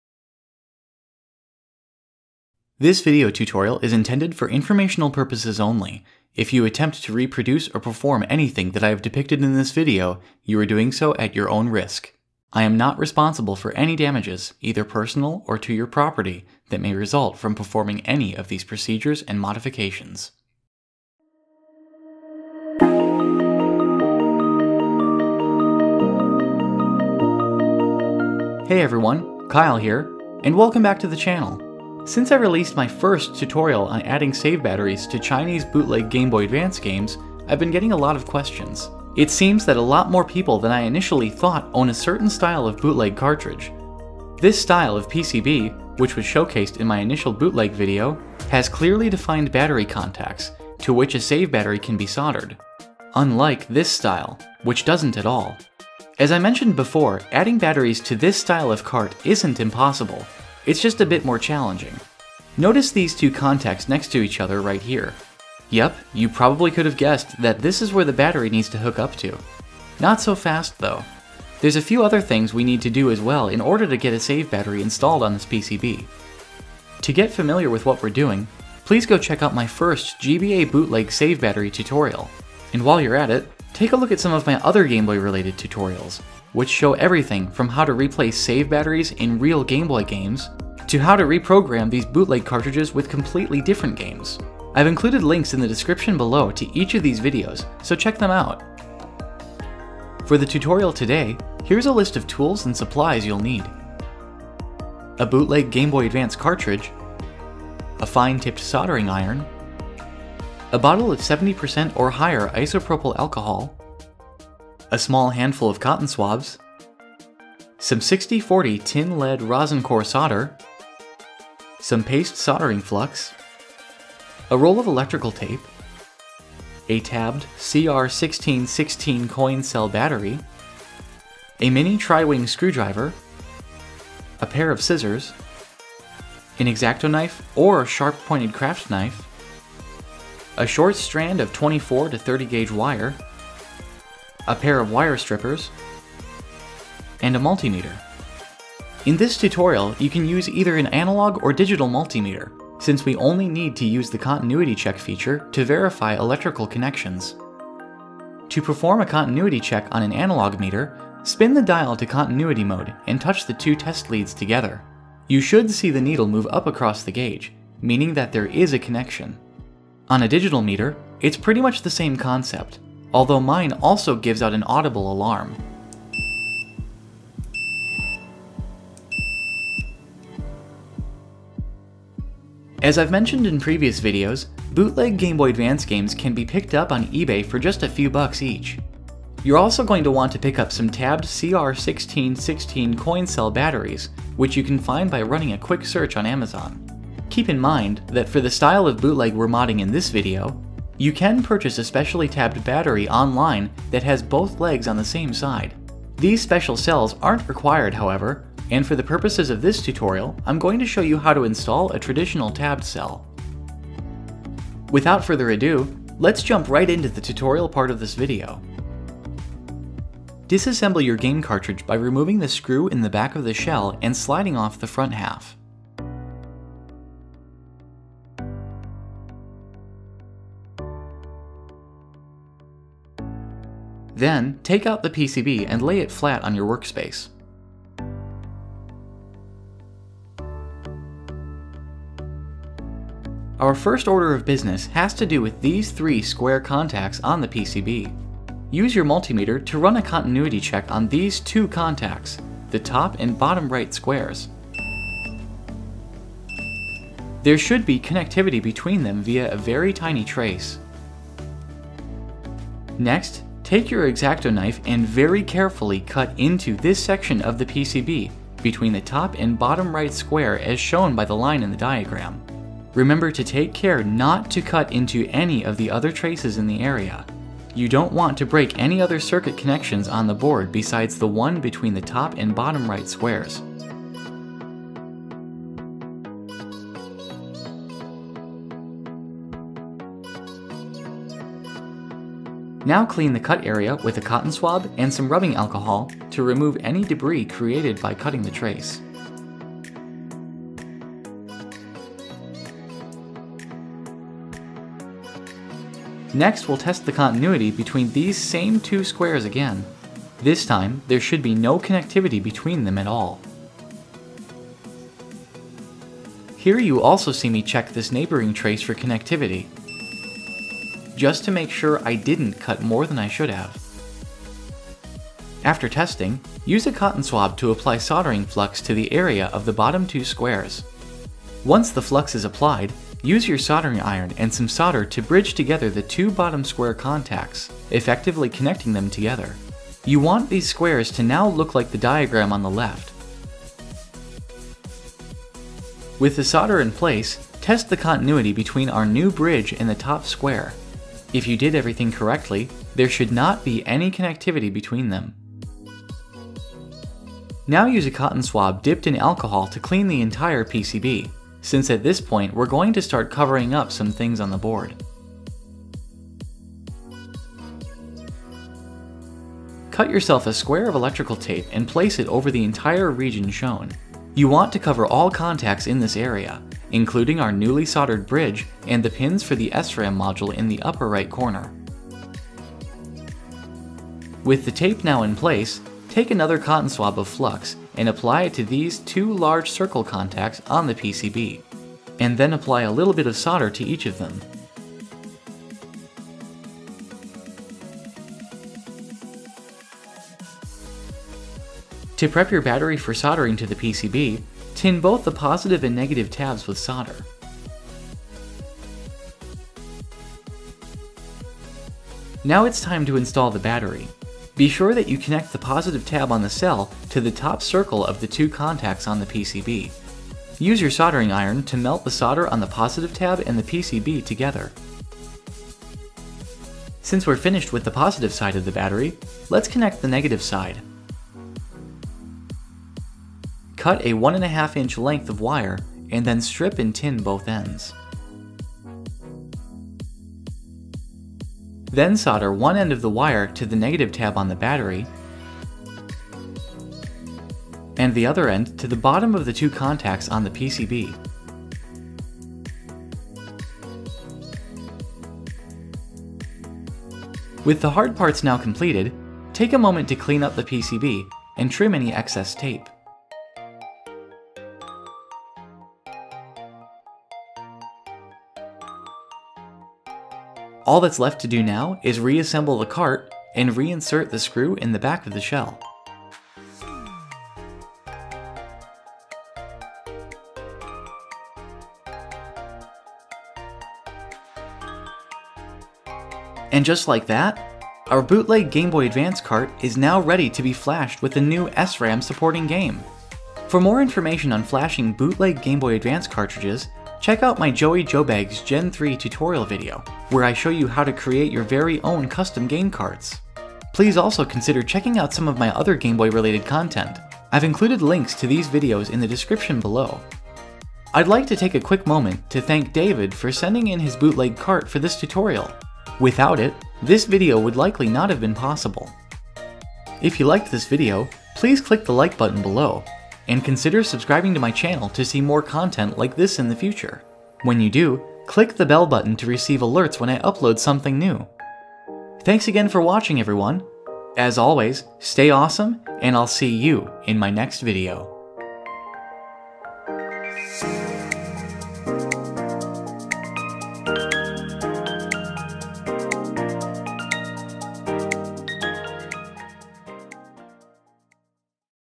Add a Save Battery to ANOTHER Bootleg GBA Cart!
With so many different versions of bootleg Gameboy Advance cartridges out in the wild these days, I decided to do a tutorial on adding a save battery to one of the [surprisingly] more common ones!  This tutorial is a bit more complicated than the last, but with this guide it should now be easy enough for even a beginner in electronics to tackle!